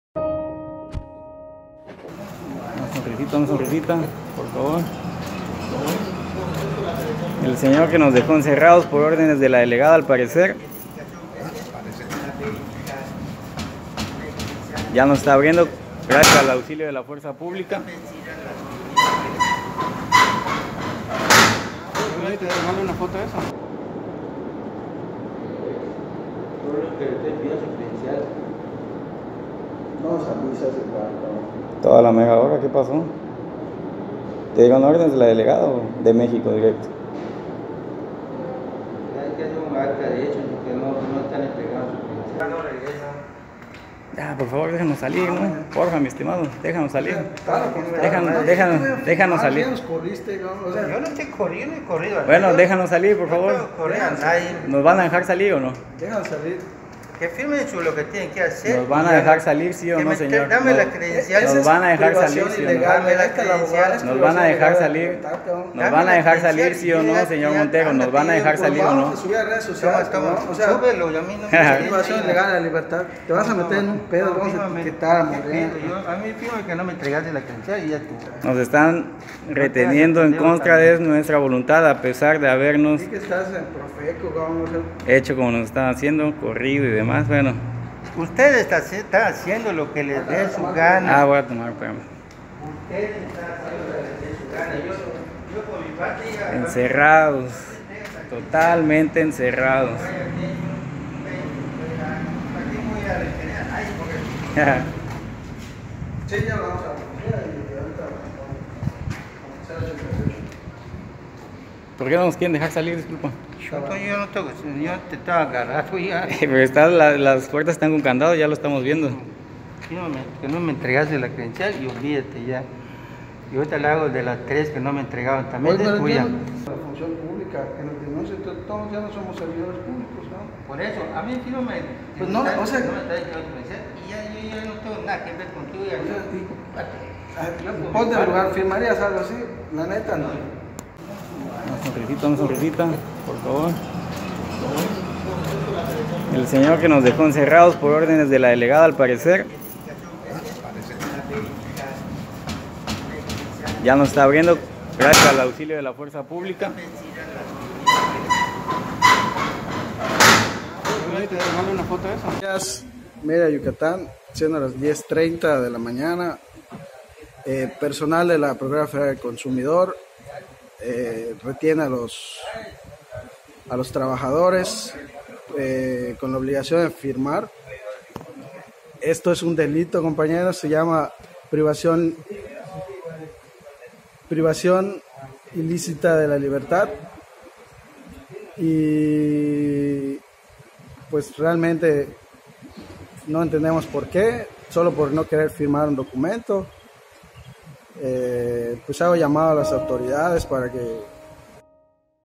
Estalla bronca en la Profeco: delgada ordena encerrar a empleados
Estalla bronca en la delegación de la Profeco en Mérida. Por instrucciones de la delegada, Sisely del Rosario Burgos Cano, encierran a empleados para pedirles su credencial al igual que su renuncia.